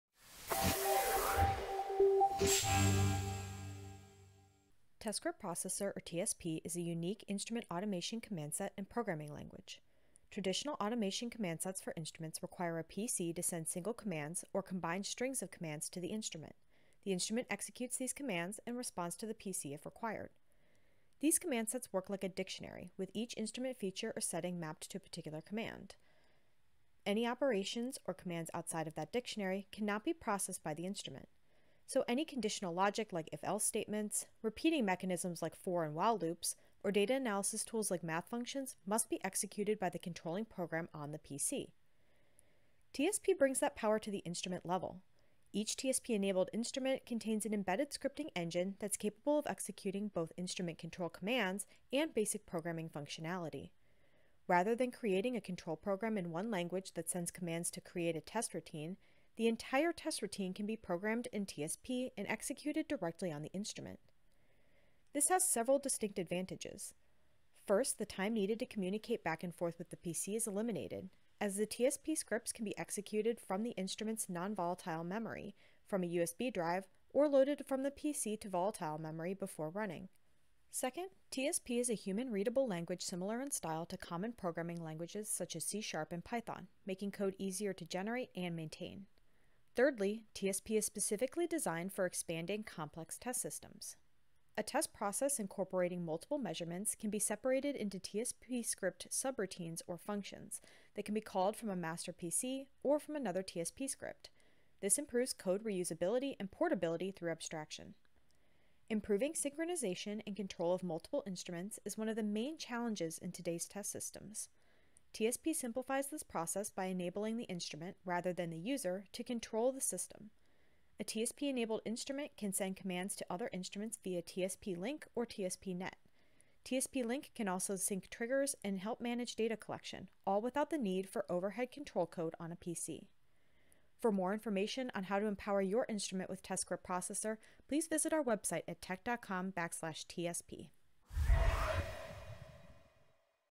TSP for Test Automation: What is TSP?
Test Script Processor or TSP-enabled instruments have an embedded scripting engine that can control the instrument and perform basic programming.